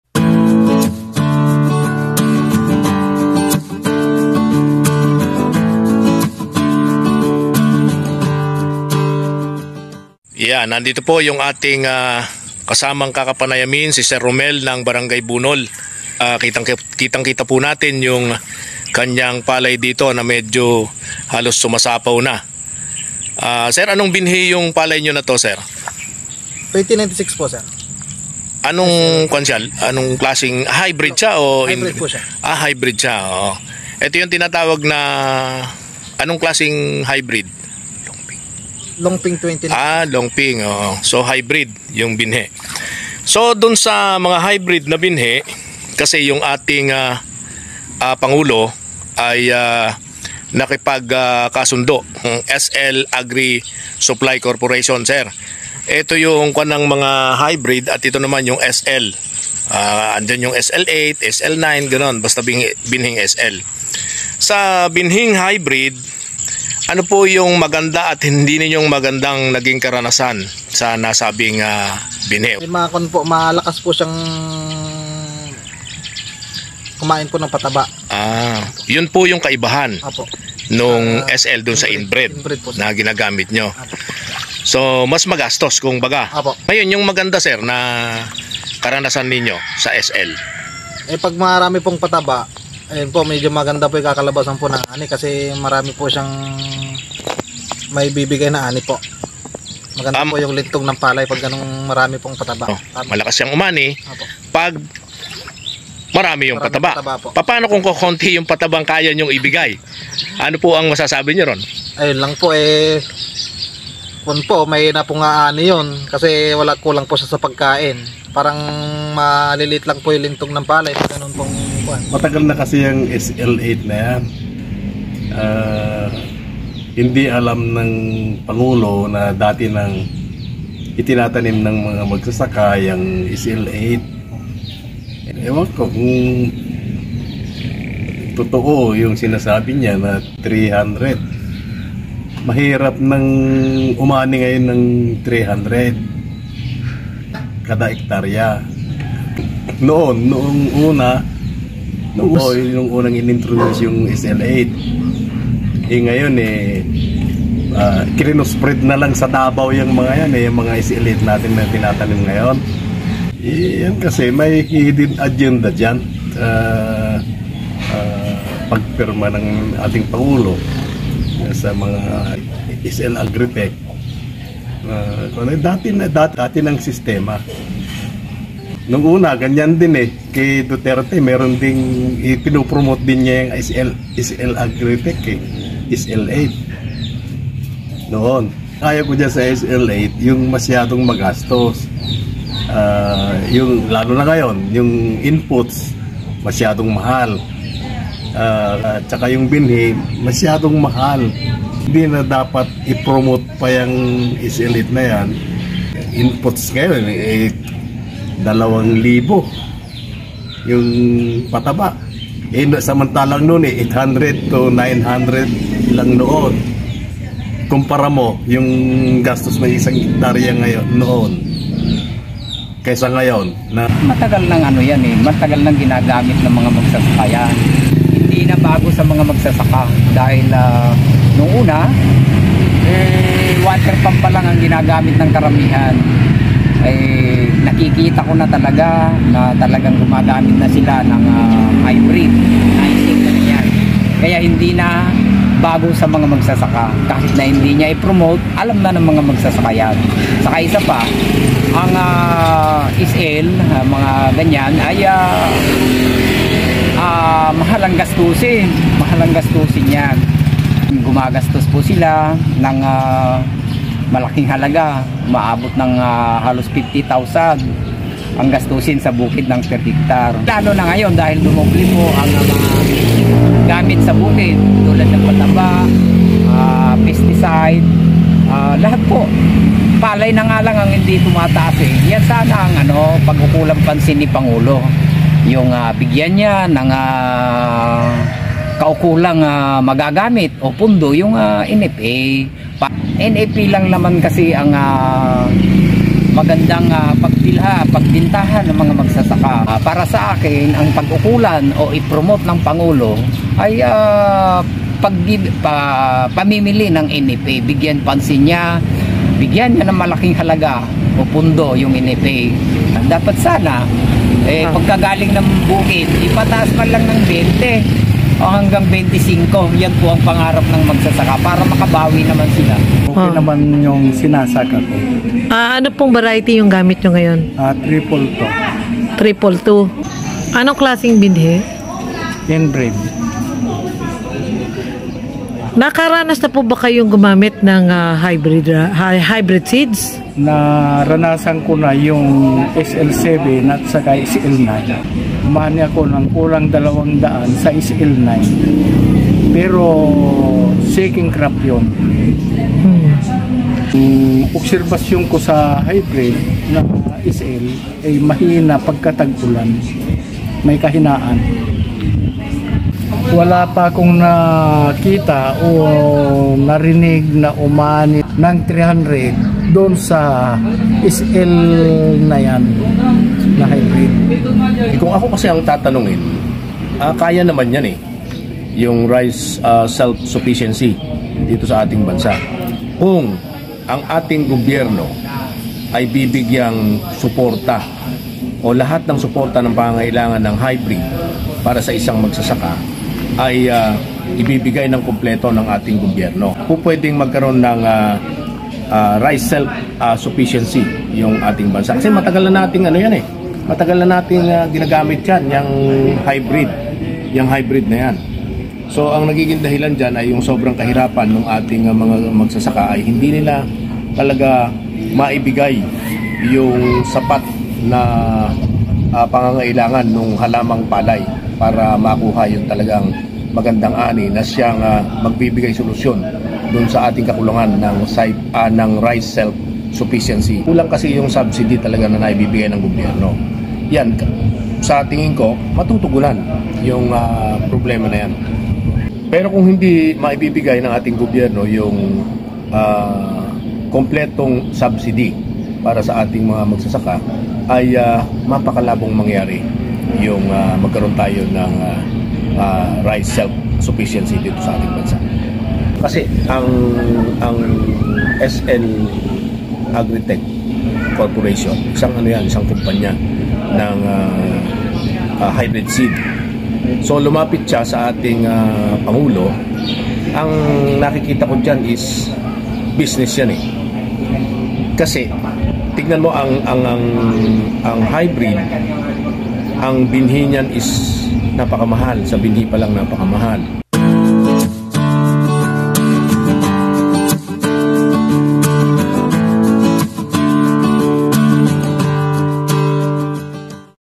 Hybrid Seeds ang itatanim ngayong 2023?
Inaprubahan ni Pangulong Ferdinand ‘Bongbong’ Marcos Jr. ang rekomendasyon na paggamit ng hybrid rice upang mapataas ang ani ng mga magsasaka.

Nakipagpulong si PBBM sa mga opisyal ng SL Agritech Corporation (SLAC) na pinangunahan ng kanilang Chairman at Chief Executive Officer Henry Lim Bon Liong kasama ang ilang magsasaka sa Central Luzon upang pagusapan hybteed seeds na umano'y mag-aangat sa industriya ng palay para makamit ang rice self sufficiency ng bansa.

Nasa 1.90 million hectares ang  target areas para pagtaniman ng hybrid na butil ng palay sa loob ng apat na taon.

Samantala, sinabi ng grupong Kilusang Magbubukid ng Pilipinas (KMP) na hindi kailangan ng hybrid seeds para makamit ng bansa ang rice self-sufficiency.

Ayon kay KMP Chairman emeritus Rafael Mariano, mas disadvantageous sa mga magsasaka sng hybrid seeds dahil sa “genetic uniformities,” kung saan mas malaki aniya ang malulugi kung tatamaan ng peste, o virus ang mga sakahan. Mas mahal rin aniya ang hybrid seeds, kaya’t mababaon din umano sa utang ang mga magsasaka.

Mas alam ng mga magsasaka kung anong binhi ng palay ang matatag, hindi madaling dumapa at lumalaban at hindi kayang pasukin ng mga sakit.

Inalam sa ilang lider- magsasaka sa Guimba kung ano ang advantages at disadvantages ng hybrid rice.